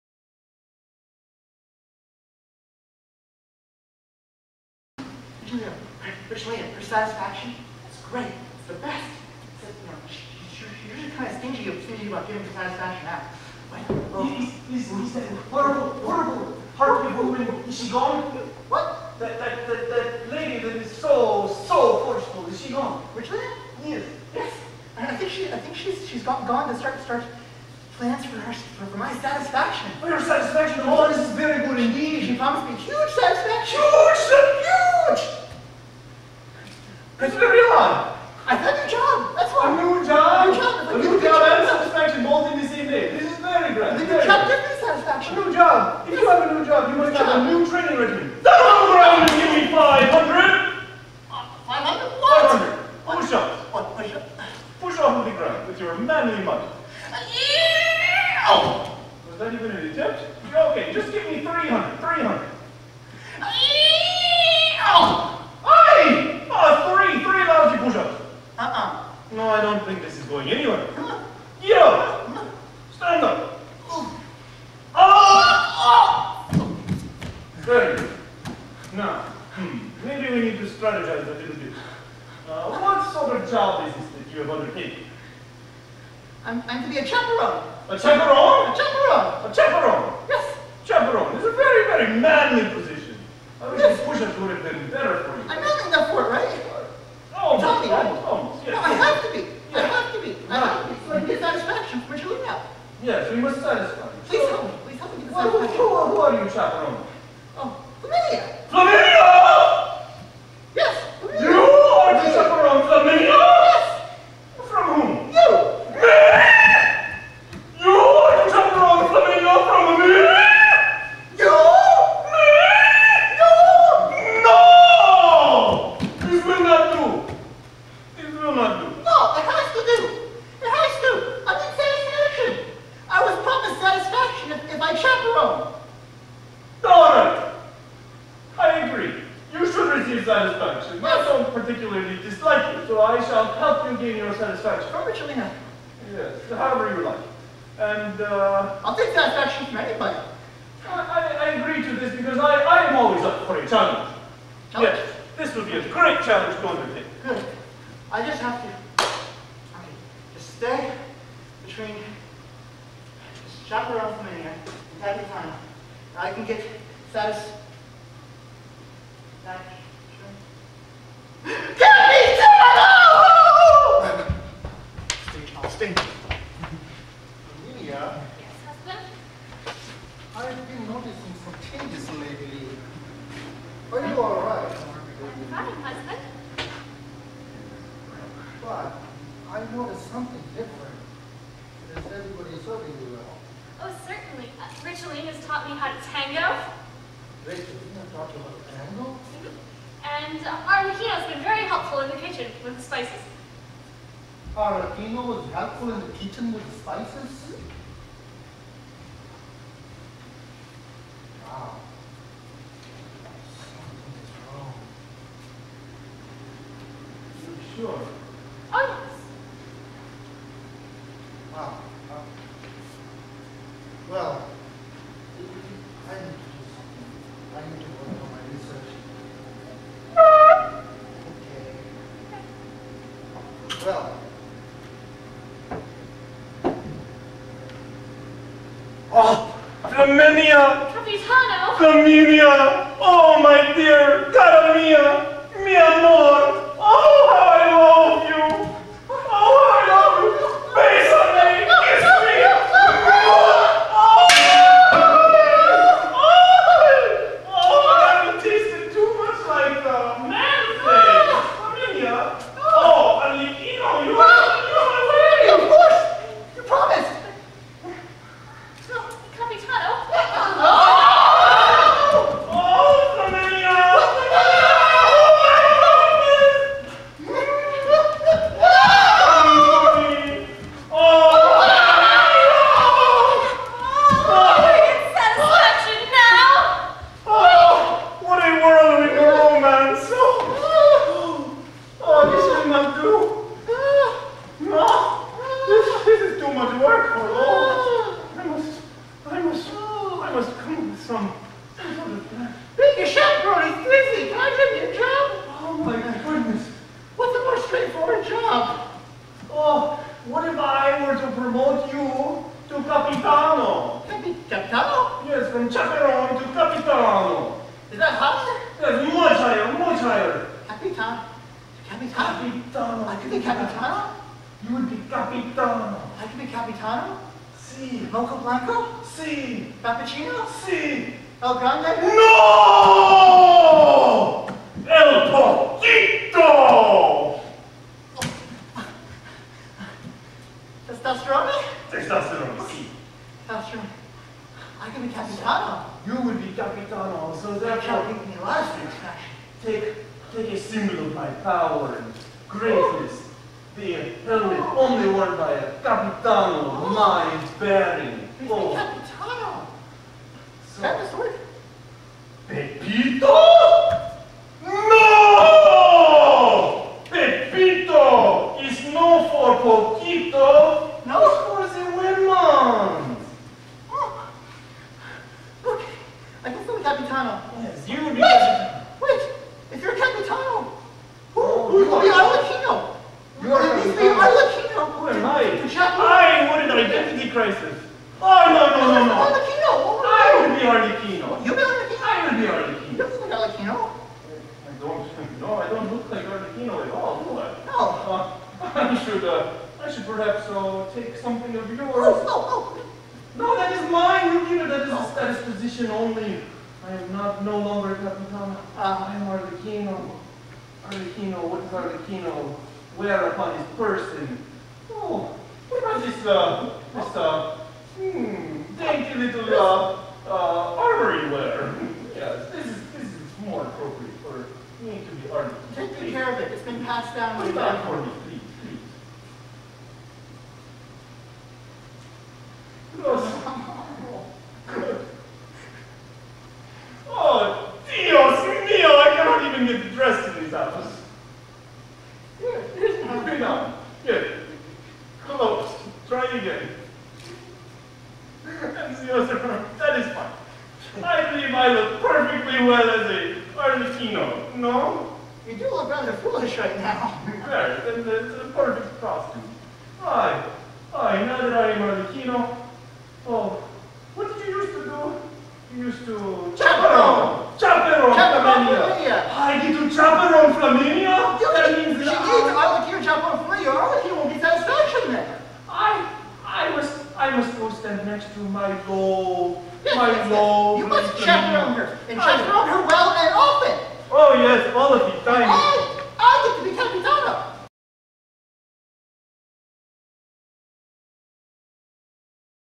The Horoscope (final dress) - Part 5 of 6
The first full three-act commedia play by The Confused Greenies! Dottore Gratiano has read his fortune in the stars and it predicts he will soon become a cuckold! Luckily, he has sent for help from Capitano Testosteroné, a man famously known as a master of cuckoldry. He might actually be the answer that both Dottore's wife, Flaminia, and their maid, Ricciolina, have been looking for. But can Arlecchino too find his own kind of satisfaction?

This final open dress rehearsal performed on April 20, 2008 in Strosacker Auditorium at Case Western Reserve University.